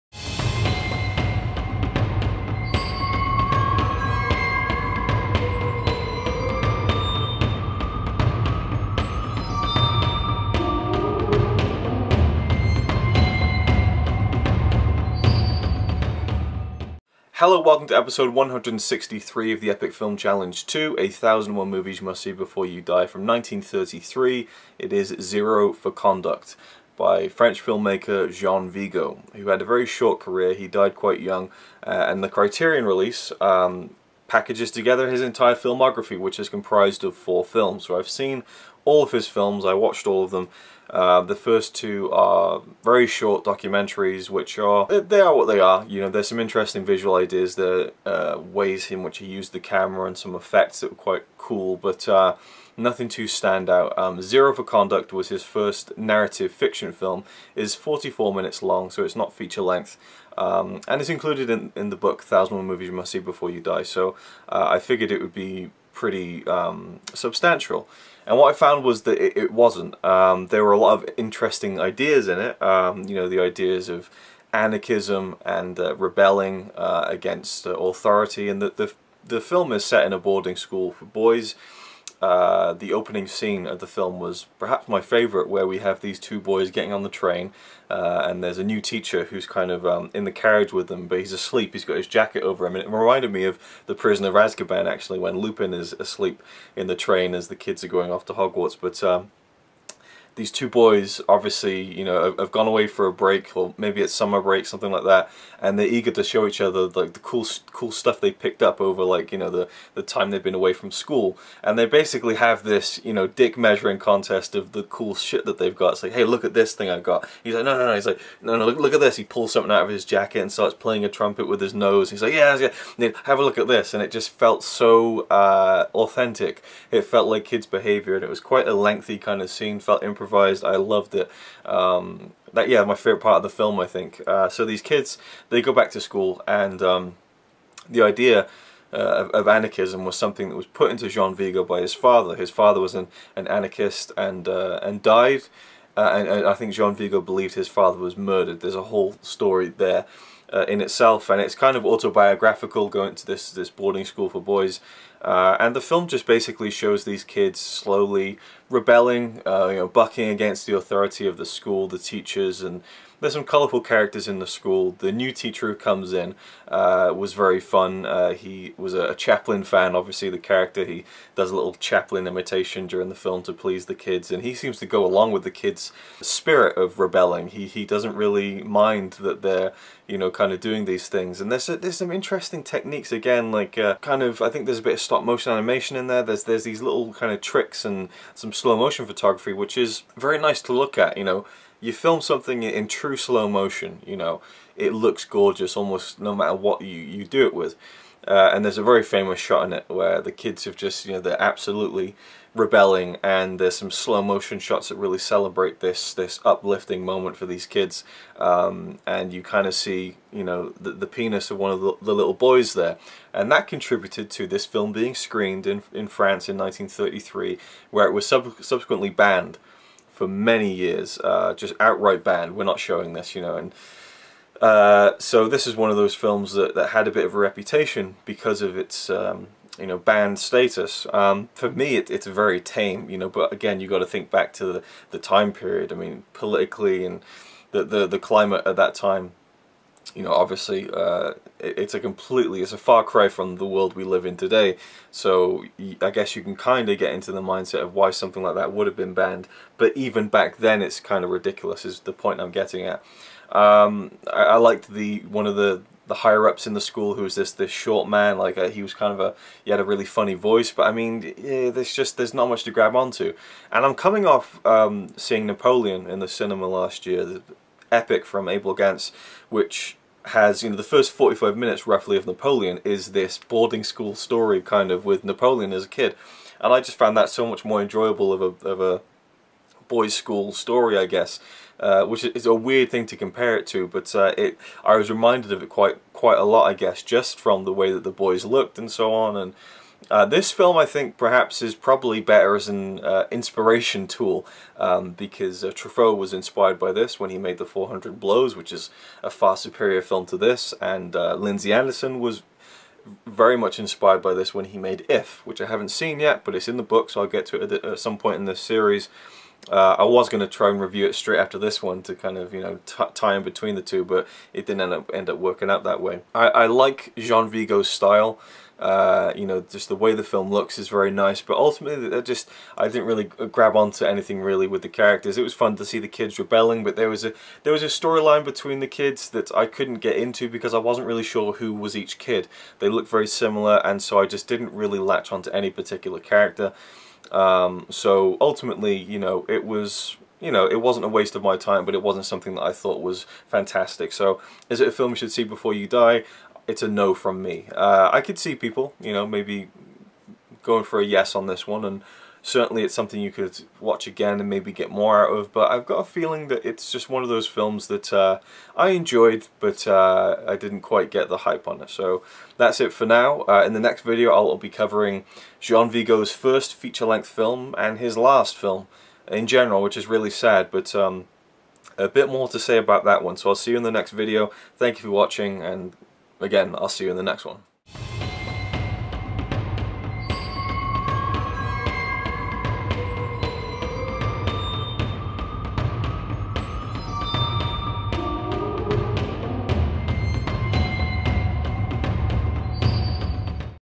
EFC II #163 - Zero for Conduct (1933)
The Epic Film Challenge II
#163 - Zero for Conduct (1933)
Dir. Jean Vigo
Starring: Gérard de Bédarieux, Louis Lefebvre, Gilbert Pruchon, Coco Golstein, Jean Dasté, Léon Larive

The one hundred and sixty-third installment of the Epic Film Challenge II, Jean Vigo's initially banned short film about a boys boarding school, Zero for Conduct.

July 2017